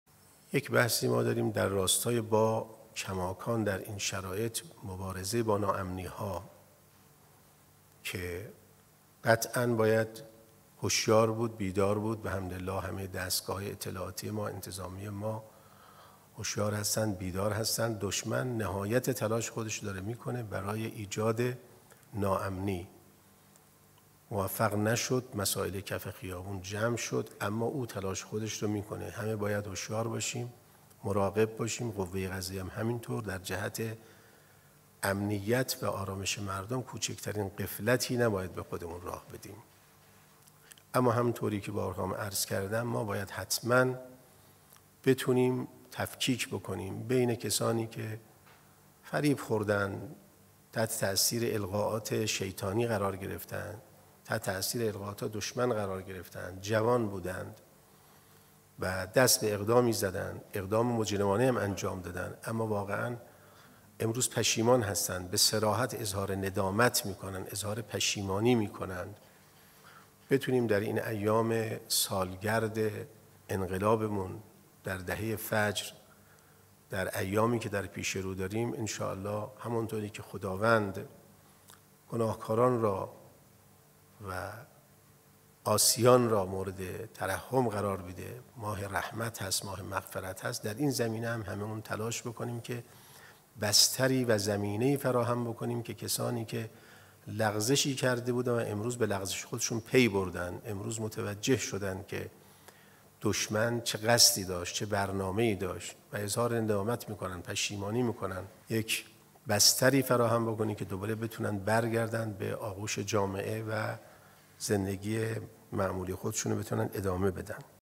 اژه‌ای: تلاش داریم نادمان اتفاقات اخیر آزاد شوند
رئیس قوه قضاییه: در تلاش هستیم در ایام جشن و سرور پیش‌رو بستری را برای عناصری که در جریان اتفاقات اخیر لغزش کردند و اکنون نادم هستند فراهم آوریم تا بتوانند به آغوش جامعه بازگردند.